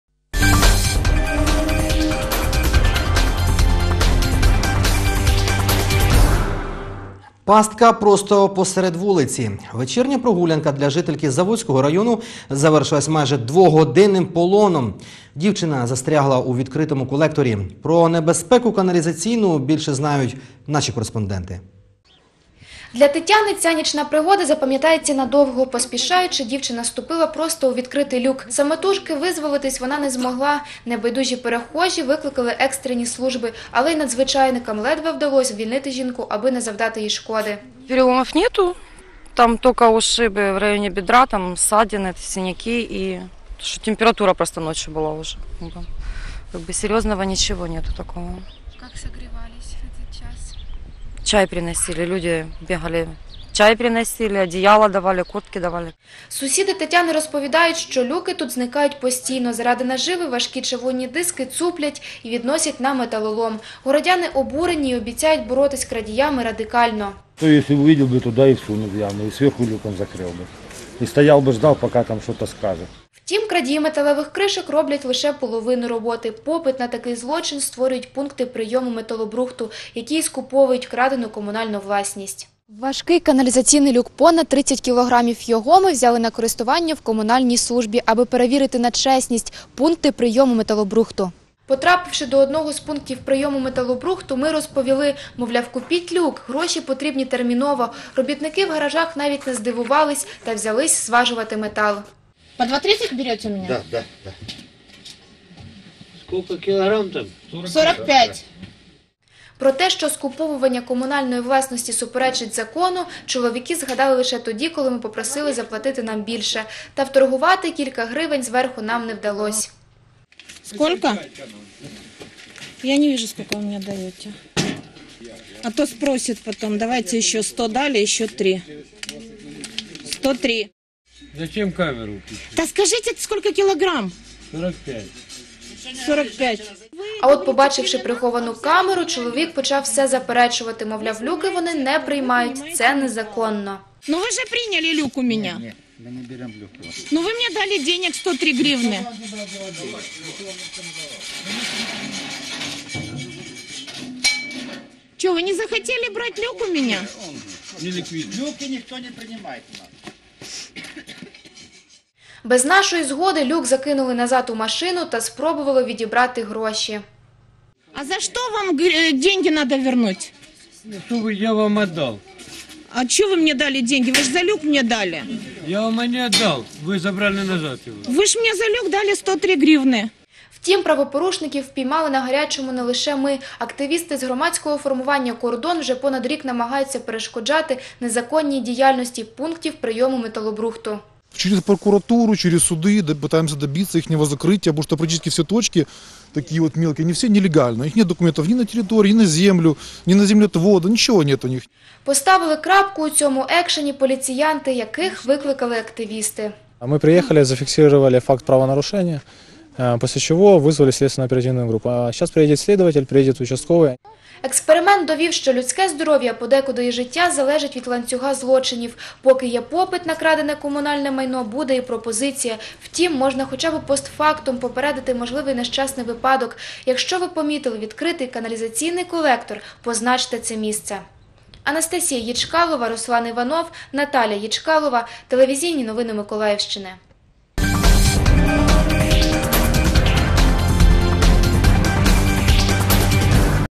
ТНМ сюжет - люк 02.02.2016
Вечірня прогулянка для жительки Заводського району закінчилась полоном.

Автор: Анастасія Єчкалова.
Оператори: Руслан Іванов, Наталя Єчкалова.